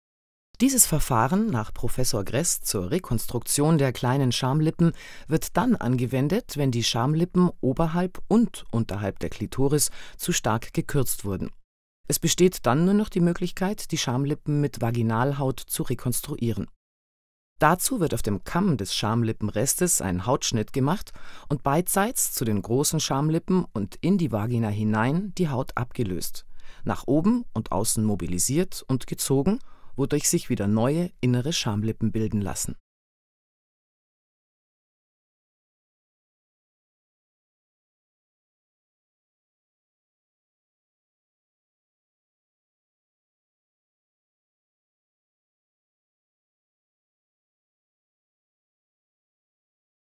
Schamlippenrekonstruktion - Rekonstruktion der kleinen Schamlippen | Prof. Dr. Gress
Rekonstruktion der kleinen Schamlippen nach Prof. Gress durch Vaginalhaut. Dazu wird die Haut in die Vagina hinein und nach seitlich abgelöst und zu einem Wall zu neuen inneren Schamlippen geformt. Dieses Verfahren kommt in Betracht, wenn oberhalb der Klitoris kein Gewebeüberschuss besteht, den man für die Rekonstruktion verwenden könnte.